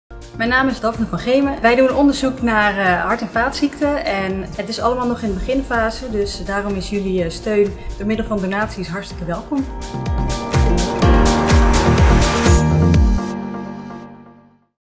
Proefdiervrij bloedvatonderzoek - help mee!
Dit is onze kans om heel veel proefdieren te redden! Er wordt gewerkt aan een ongelooflijk goed proefdiervrij onderzoeksmodel voor hart- en vaatziekten. Dit móet echt verder ontwikkeld worden –en dat kan met jouw hulp. Geef nu ook een bijdrage en help de proefdieren!